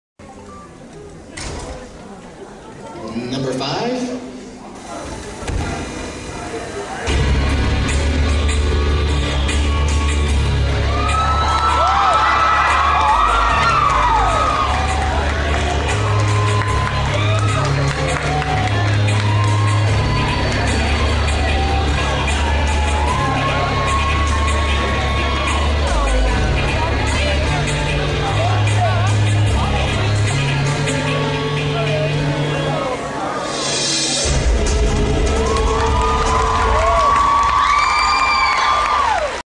Sac Anime Summer 2009 - W05 Albert Wesker Resident Evil
Video of Entry W05, cosplay walk-on of Albert Wesker Resident Evil performed at Sac Anime Summer 2009.  (Permission granted)